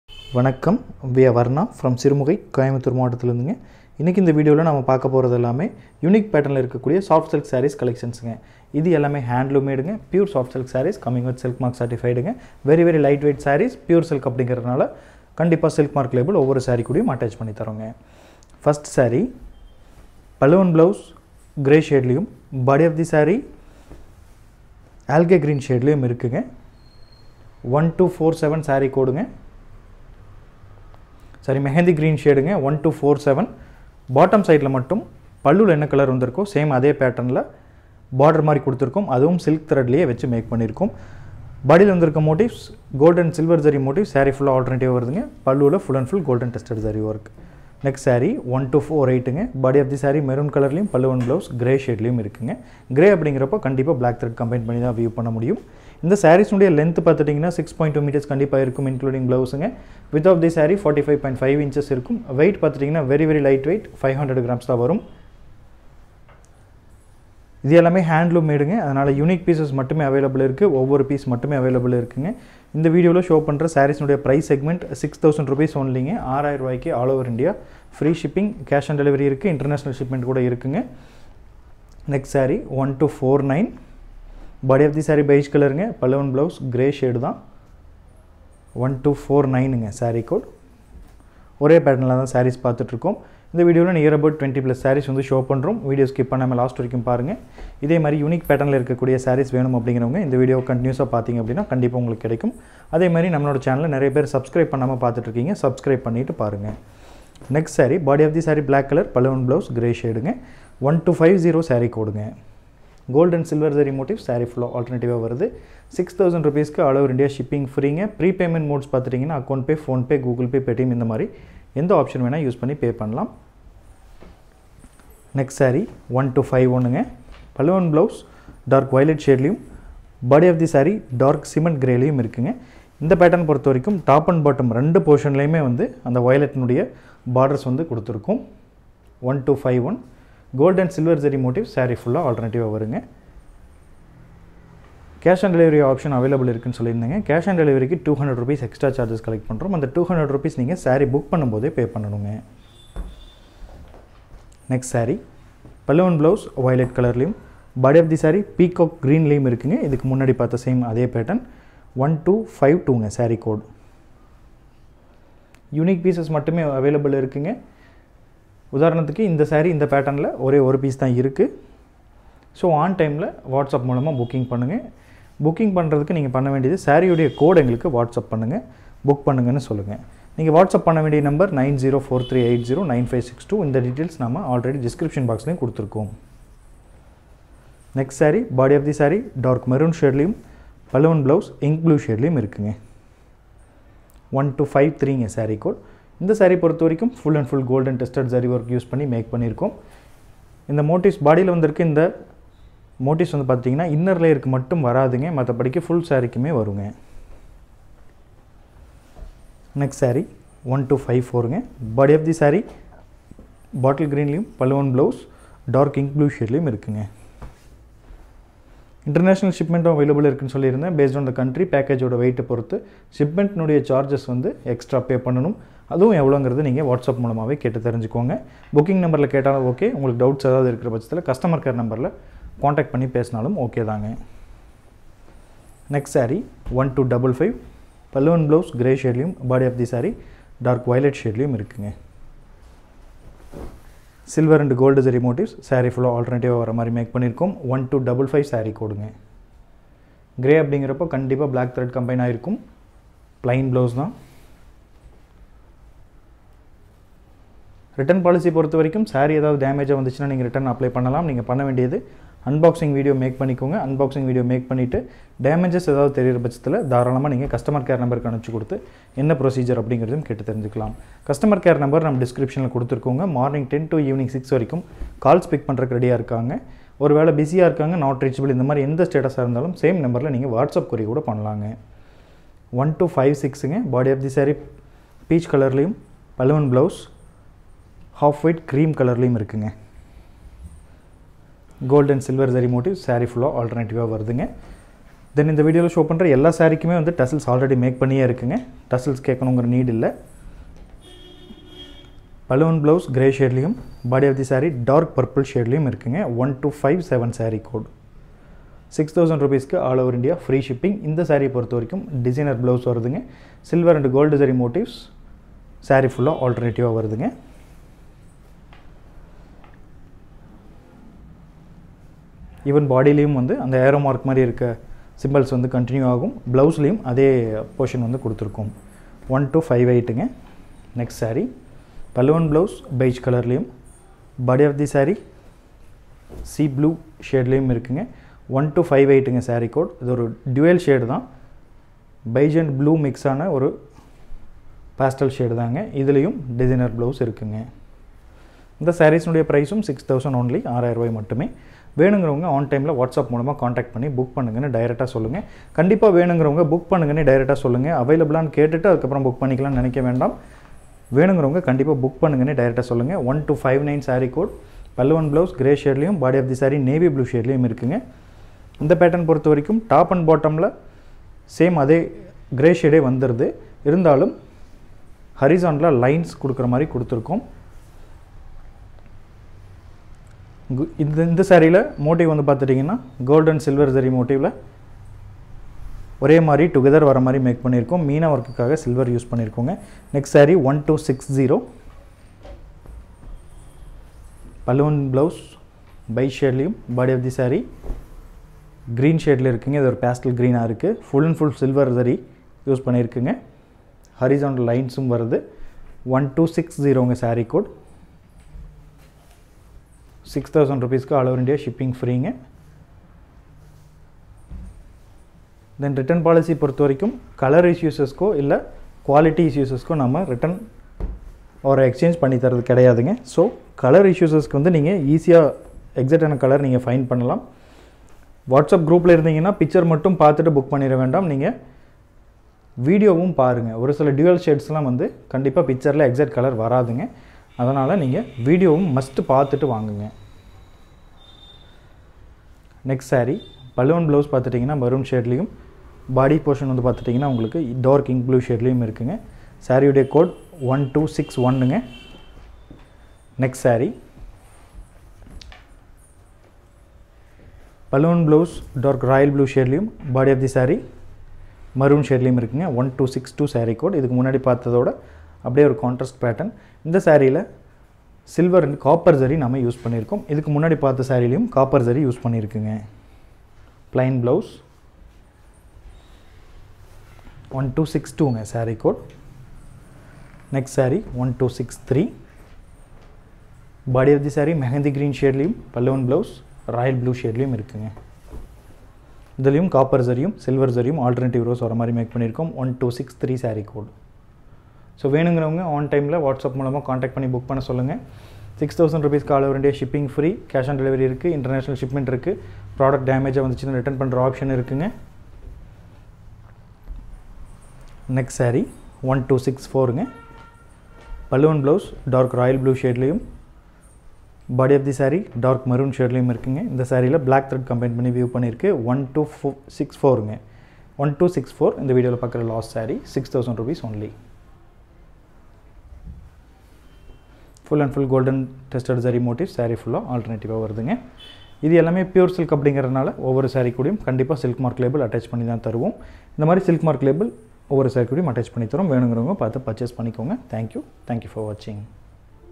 unique pattern soft silk sarees | soft silk sarees with price | varnaa soft silk sarees
unique pattern soft silk sarees @ 5999/-

Handloom made |  Pure Silk sarees | 1.3/4 warp |  Light Weight | with Silk mark | Cash on delivery | International shipping

(if full ask us in whatsapp for new link)

Varnaa
11/137c, cheran nagar,
Annur Road
Sirumugai ,
Coimbatore dt.
Tamil nadu